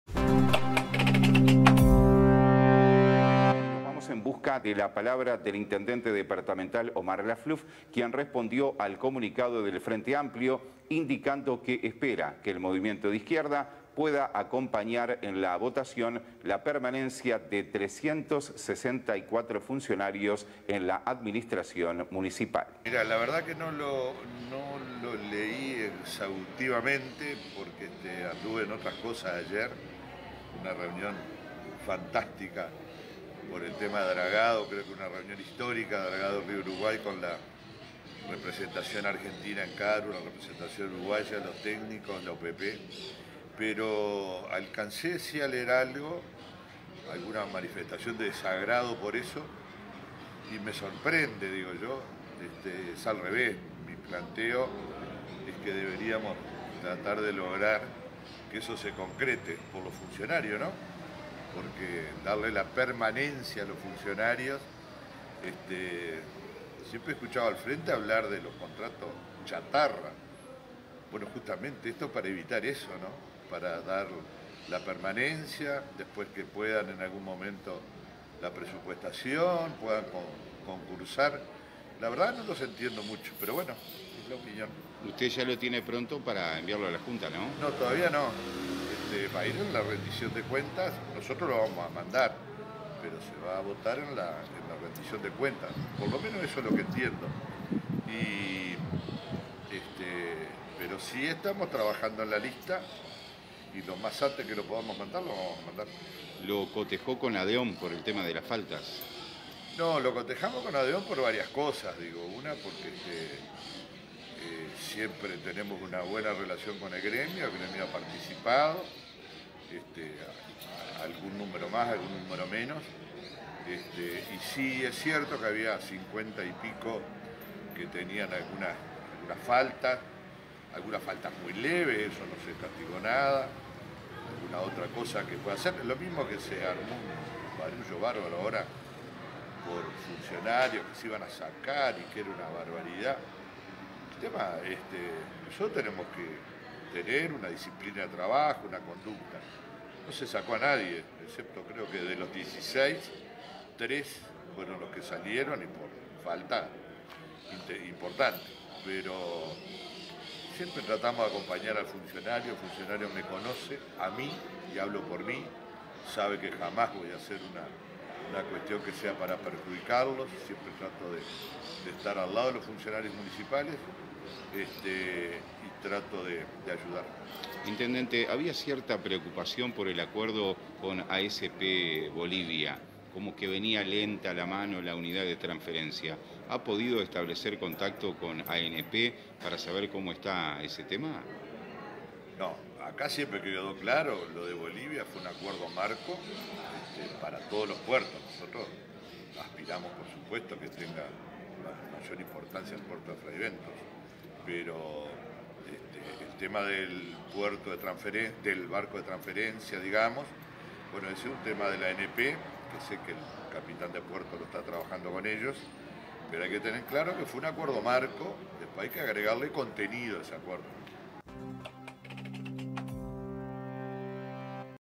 POLÍTICA: Respuesta de Omar Lafluf al Frente Amplio | 𝗧𝗘𝗟𝗘𝗥𝗘𝗚𝗜𝗢𝗡𝗔𝗟
El Intendente Dr. Omar Lafluf respondió al comunicado del Frente Amplio de Río Negro, indicando que espera que la fuerza política de izquierda pueda acompañar en la votación la permanencia de 364 funcionarios en la Administración Municipal.

𝗧𝗘𝗟𝗘𝗥𝗘𝗚𝗜𝗢𝗡𝗔𝗟 | 𝗥𝗶́𝗼 𝗨𝗿𝘂𝗴𝘂𝗮𝘆 𝗧𝗩 𝗖𝗮𝗻𝗮𝗹 𝟭𝟮 𝗙𝗿𝗮𝘆 𝗕𝗲𝗻𝘁𝗼𝘀